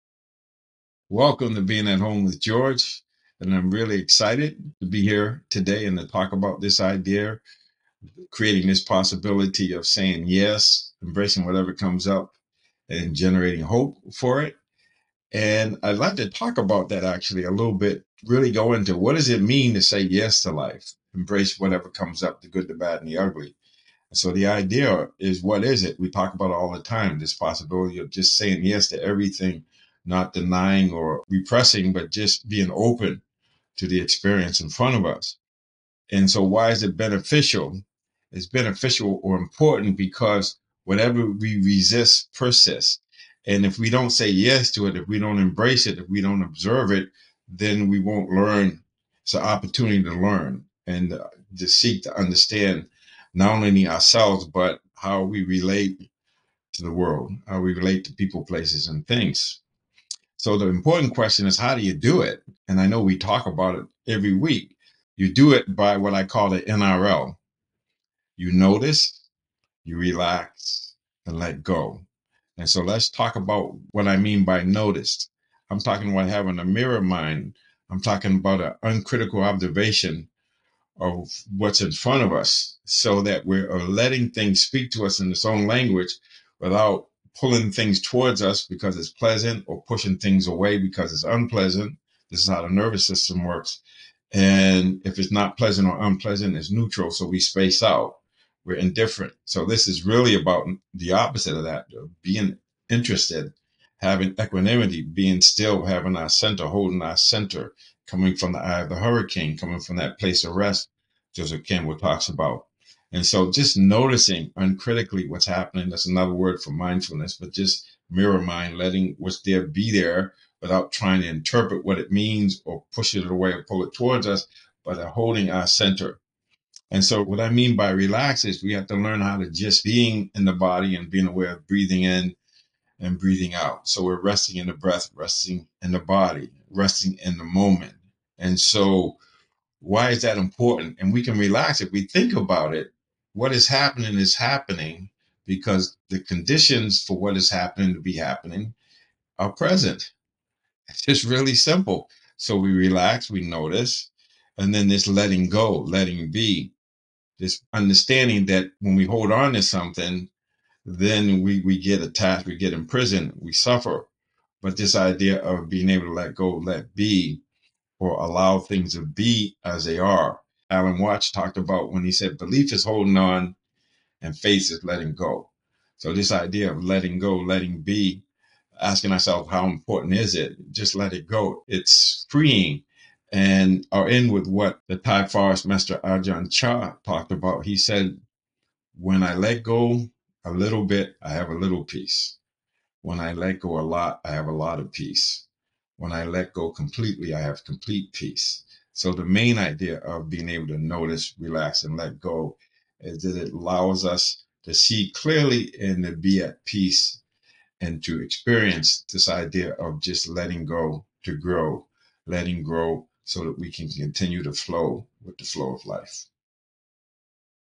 Notice + Relax + Let Go = A Technique To Say Yes To Life / "At Home with George” - August 25, 2023
In this "At Home With George” short, mindfulness and performance expert George Mumford discusses a specific technique he uses to “say ‘Yes’ to whatever comes up”, part of the framework of each of these episodes.

George: “By notice, I'm talking about having mirror mind. I'm talking about an uncritical observation of what's in front of us so that we're letting things speak to us in their own language without pulling things towards us because they're pleasant or pushing things away because they’re unpleasant. This is how the nervous system works. And if it's not pleasant or unpleasant, it's neutral so we space out or we're indifferent."

George also quotes Thai forest master Ajahn Chah and philosopher Alan Watts. And as always, he frames the talk around the possibility of both saying yes to whatever comes up, including things that are unpleasant to embrace, and generating the hope.

Ajahn Chah: “When I let go a little bit, I have a little peace. When I let go a lot, I have a lot of peace. When I let go completely, I have complete peace."

More information on George and the resources he offers can be found